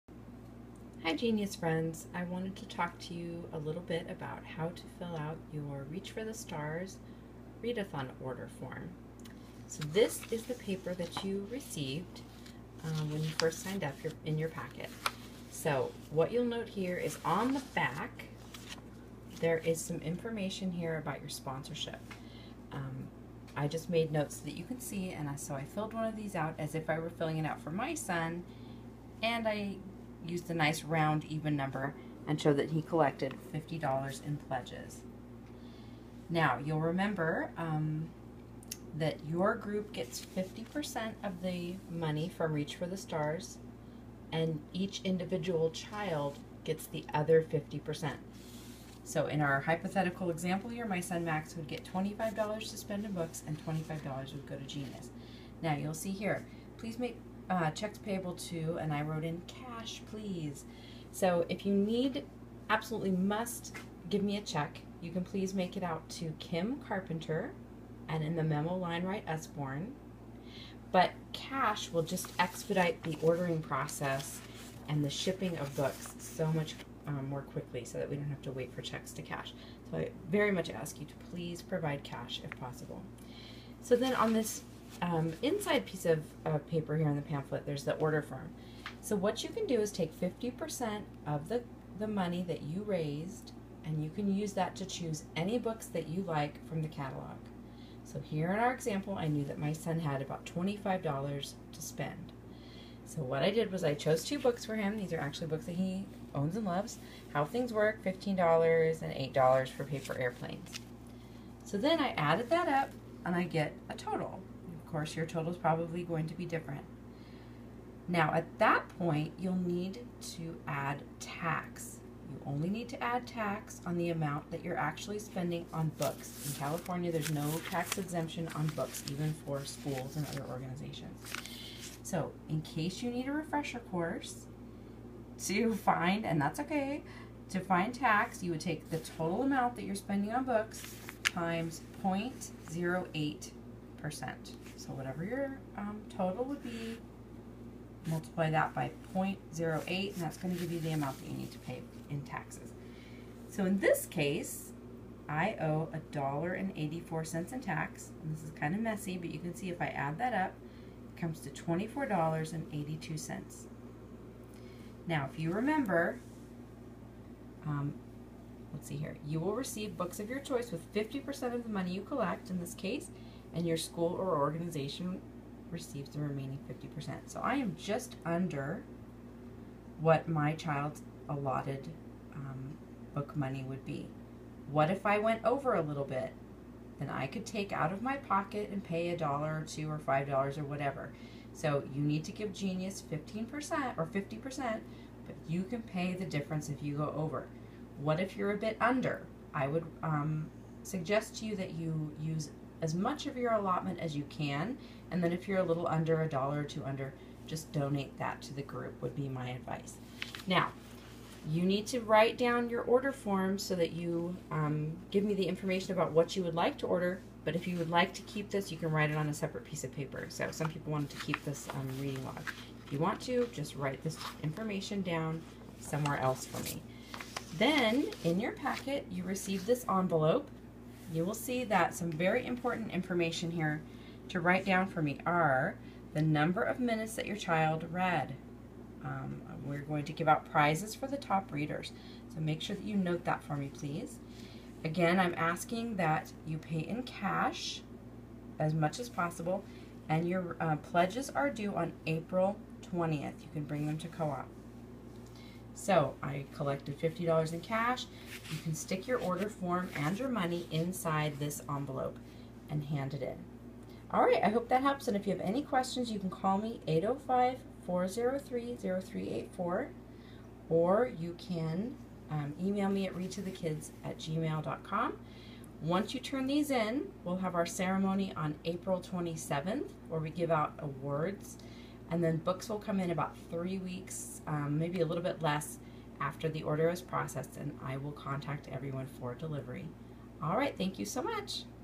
How To Order Your Books
How to order your Reach For The Stars read-a-thon books.
Questions?
To see the full catalog and choose your books, go to readtothekids.com